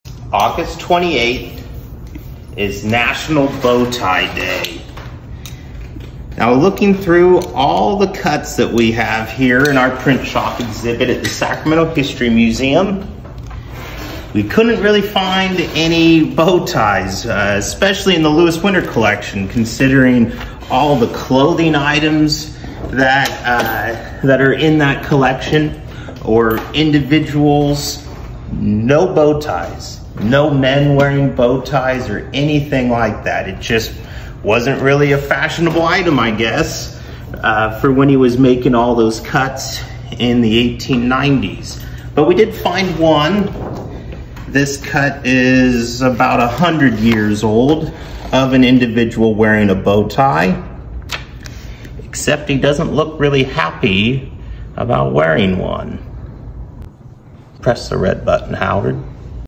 Letterpress printing for National Bow Tie Day (August 28)
Trying to find something from the print shop was difficult for today. August 28th is National Bow Tie Day.

While the Lewis Winter Collection, which is full of cuts of clothing items for catalogs in the 1890s, there are no bow ties. However, Jared found this cut, made about 100 years ago, from the Bill Gaylord Collection. The cartoon in the print doesn’t look very happy, but at least he’s dressed in his Sunday’s best!